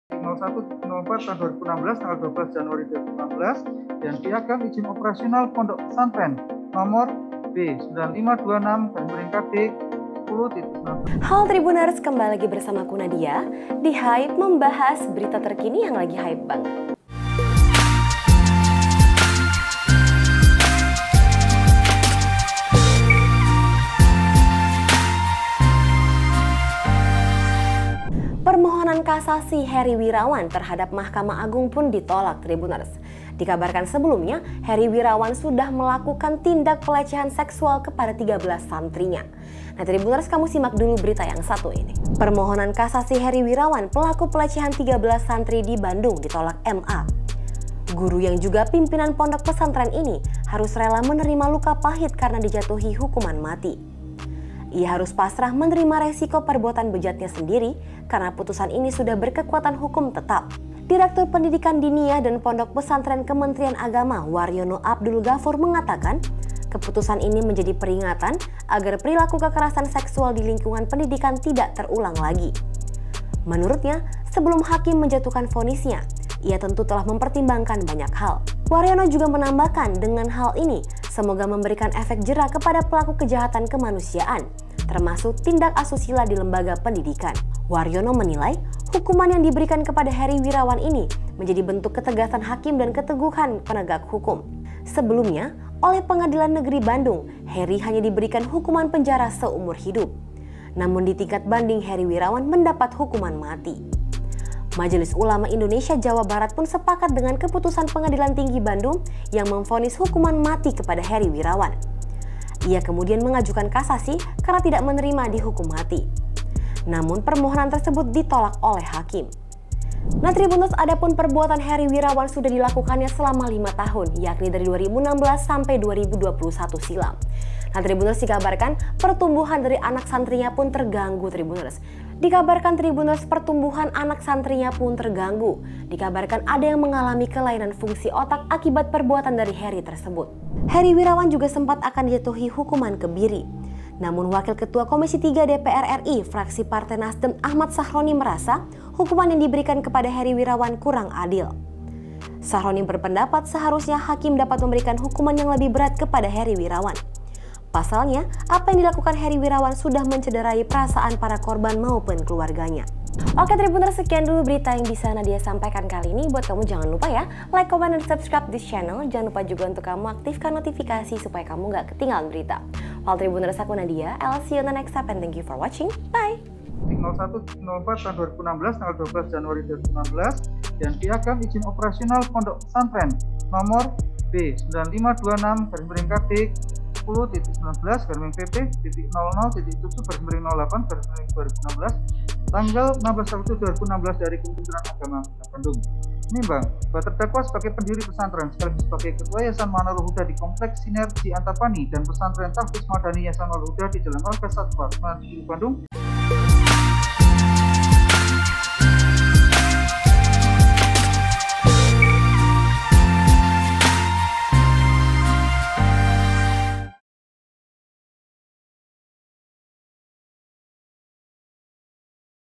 Herry Wirawan Divonis Mati, Peringatan Untuk Kasus Pelecehan
Permohonan kasasi Herry Wirawan, pelaku pemerkosa 13 santri di Bandung, ditolak Mahkamah Agung (MA).
Guru yang juga pimpinan pondok pesantren ini harus rela menerima luka pahit karena dijatuhi hukuman mati.
Ia harus pasrah menerima resiko perbuatan bejatnya sendiri karena putusan ini sudah berkekuatan hukum tetap.